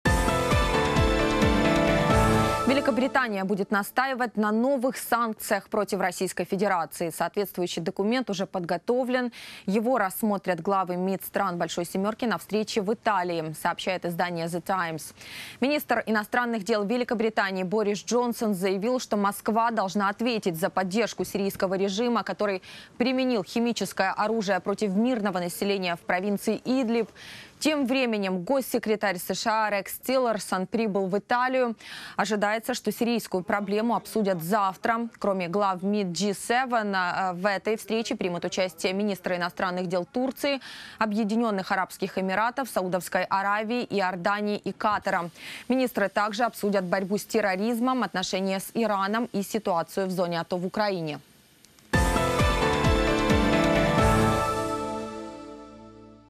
Новые санкции
Великобритания будет настаивать на новых санкциях против Российской Федерации. Соответствующий документ уже подготовлен, и его рассмотрят главы МИД стран большей семерки на встрече в Италии.